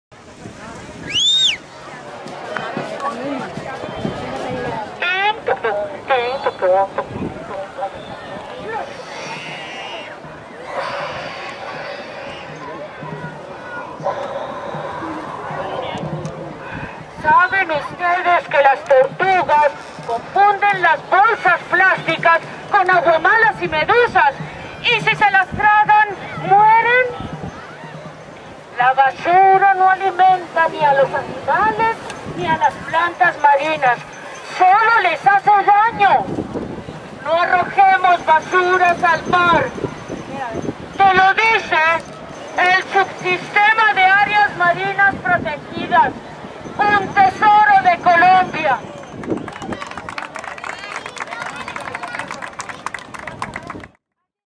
Un mensaje del SAMP a los turistas, de por qué es riesgoso arrojar basuras al mar
Por medio de representaciones teatrales en la ciudad de Santa Marta el SAMP aconseja de cómo hacer turismo responsable, y por qué es importante no arrojar basuras al mar.